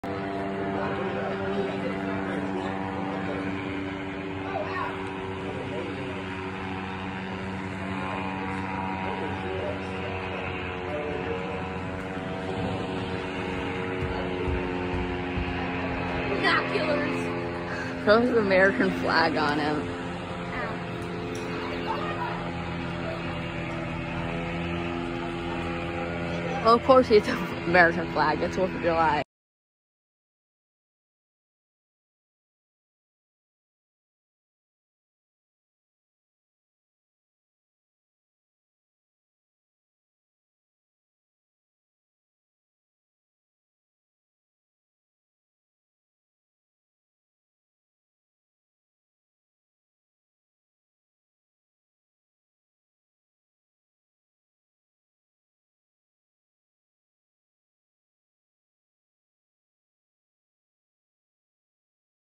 Motor fan +beach time lapse (no skateboarding Sorry)
Sorry, it’s not skateboarding can’t skateboard right now and I want to just post some videos ￼ ￼